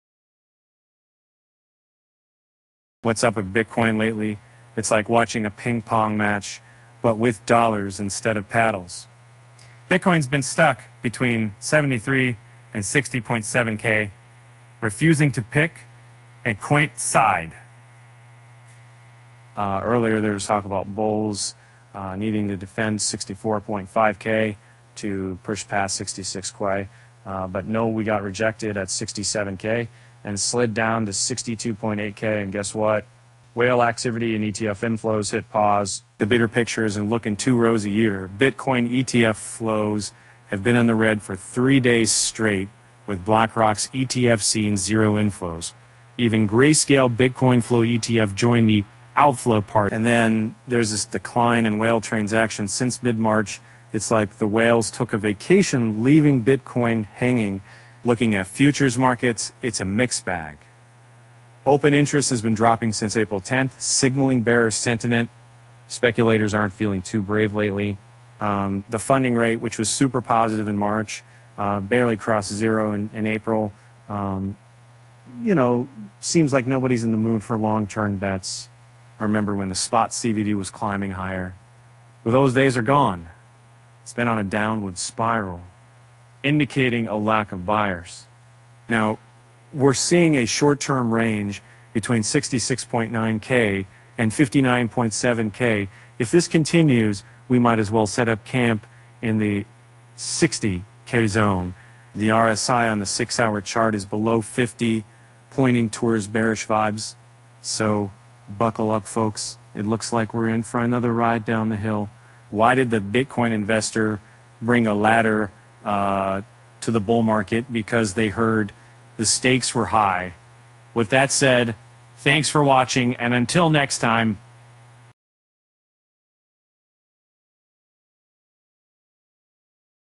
Bitcoin – $75K or $55K? Here’s where BTC will go next
What's up with Bitcoin lately? It's like watching a ping pong match, but with dollars instead of paddles. Bitcoin's been stuck between $73k and $60.7k, refusing to pick a side.